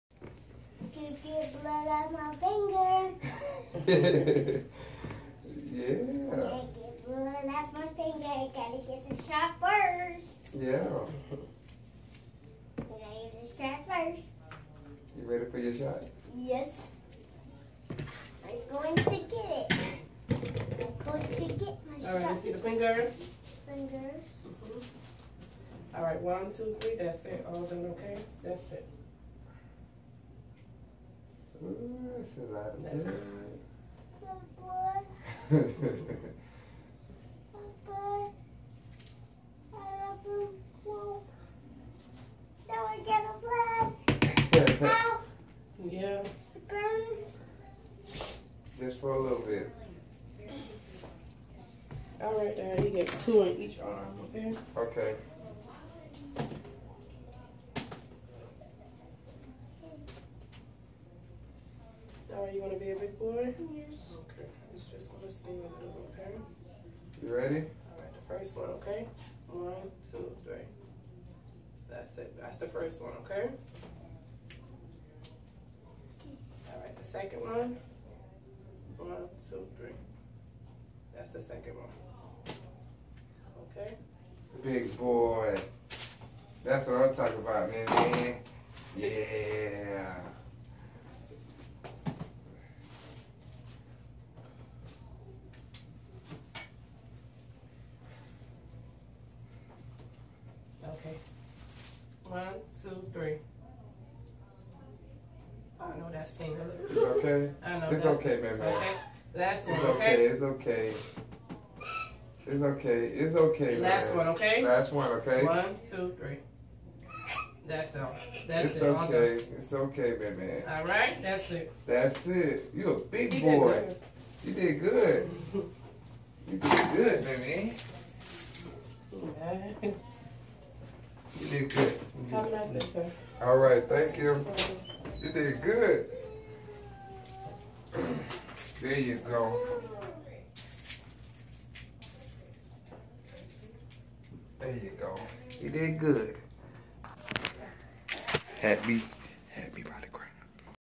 Brandon jr gets his 4 year old shots.
Brandon took his shots like a G I have never seen a kid who like to get shots.